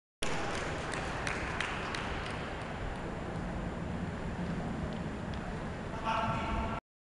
peso, secondo lancio
Campionati Italiani di prove multiple indoor ad Ancona